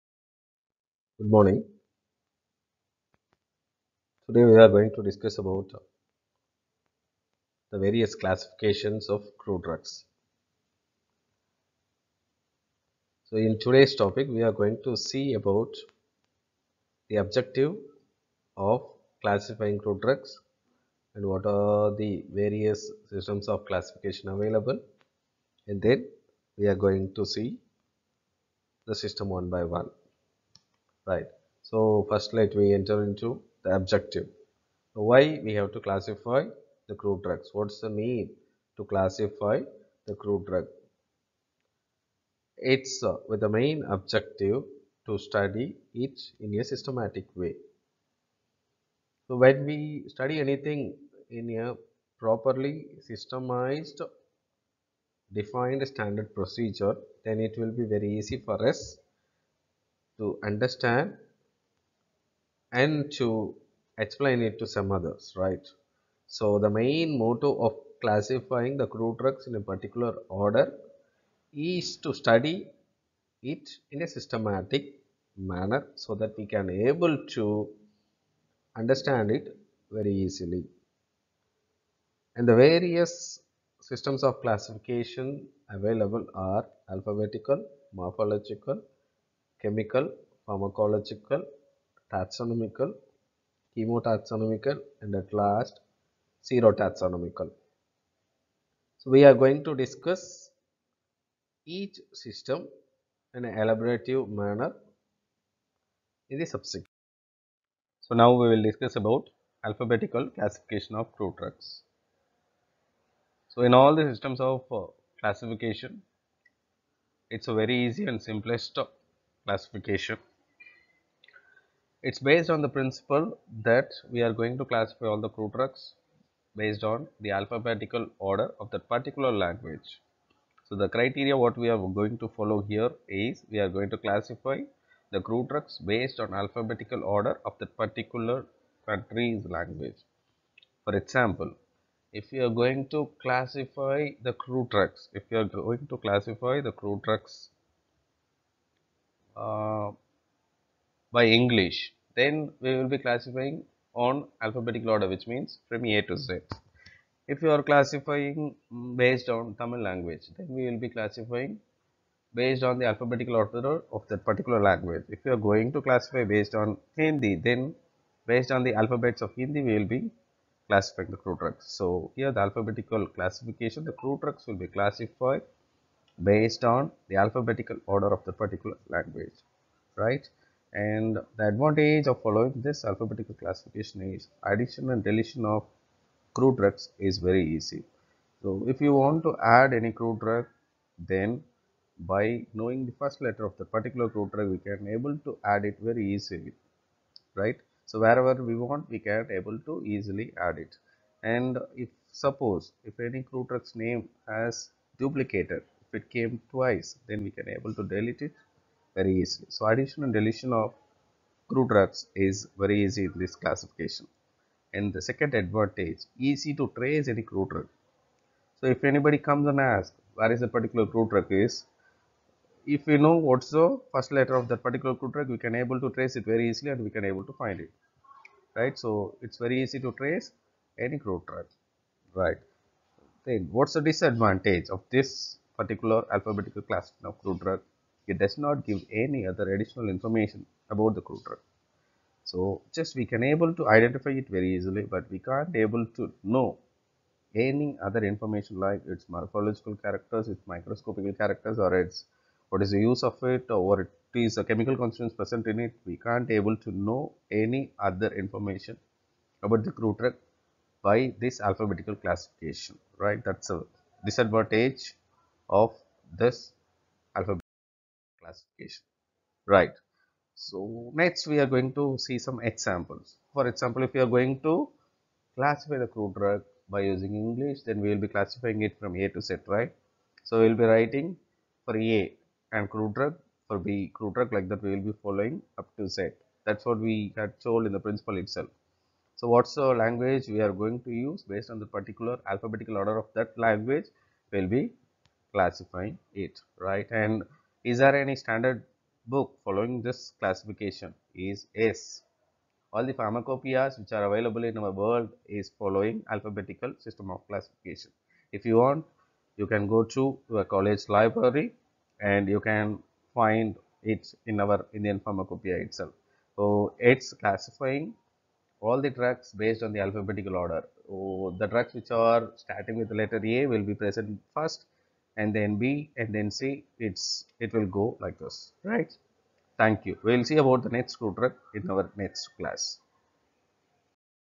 CLASSIFICATION OF CRUDE DRUGS
Introduction to Classification of crude drug + Alphabetical classification